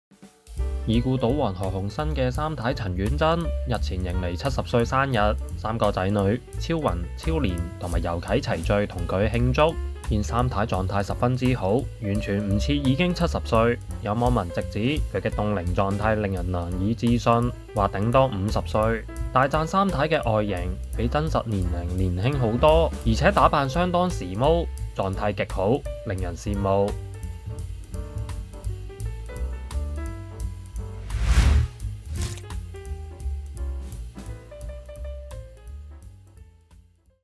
賭王三太陳婉珍70歲大壽 外貌超年輕網民謂難以置信｜01娛樂｜陳婉珍｜三房｜生日
已故賭王何鴻燊三太陳婉珍日前迎來70歲生日，三名子女超雲、超蓮及猷啟齊聚與她慶祝，見三太十分之好，完全不似已經70歲，有網民直指其凍齡狀態令人難以置信。